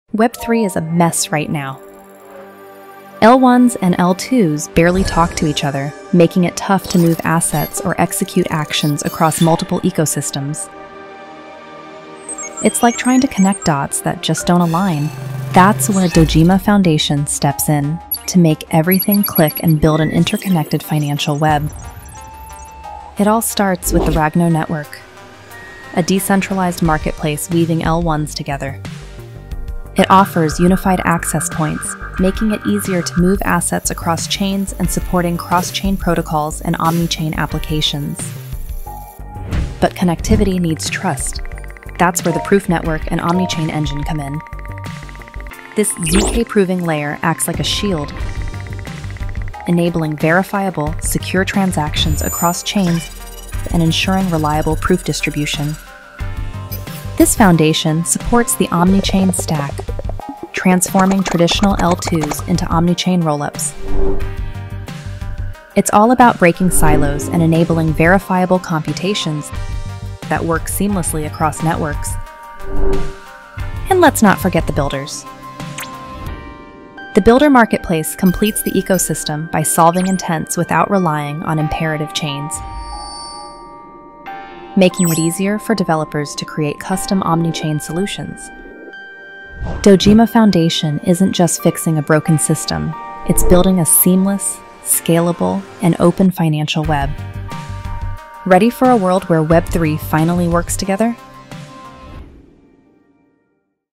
Dojima Foundation: Introducing the Omnichain Web
Join the revolution in blockchain connectivity! Dojima Foundation is proud to announce its universal framework, the Omnichain Web, a suite of innovative platforms designed to bridge the gaps between blockchains. It serves as the "Internet for Decentralized Finance," breaking down silos between Layer 1 (L1) and Layer 2 (L2) blockchains to enable seamless asset transfers, data exchange, and decentralized application (dApp) development across chains.

Explore the following websites that makes Dojima Ecosystem whole as we redefine the Web3 landscape :

Imagine a blockchain ecosystem without barriers, where data, assets, and applications move seamlessly across chains. Omnichain Web makes this a reality, unlocking true interoperability and redefining how blockchains communicate.

🚀 Core Pillars of Omnichain Web:

1️⃣ Ragno Network: The decentralized crawler that indexes all Layer 1s, forming the backbone of cross-chain connectivity.
2️⃣ Builder Marketplace: Empowering developers with low-code tools and AI integration to create next-gen dApps effortlessly.
3️⃣ Omnichain Stack: The robust technological foundation enabling fluid interoperability across the multichain ecosystem.
4️⃣ Proof Network: Securing and verifying transactions with cutting-edge zk-proof technology.
5️⃣ Omnichain Engine: The driving force behind seamless blockchain interaction.

✨ Why Omnichain Web Matters ?

We’re not just connecting blockchains—we’re building the Internet for Decentralized Finance. This is the infrastructure that powers liquidity, innovation, and limitless possibilities in Web3.

💡 Welcome to the future. Welcome to the Omnichain Web.

Join us in revolutionizing blockchain interoperability. Together, we build the connected Web3 world.
Learn more about our mission to dismantle silos and create a fluid, interconnected Web3 environment.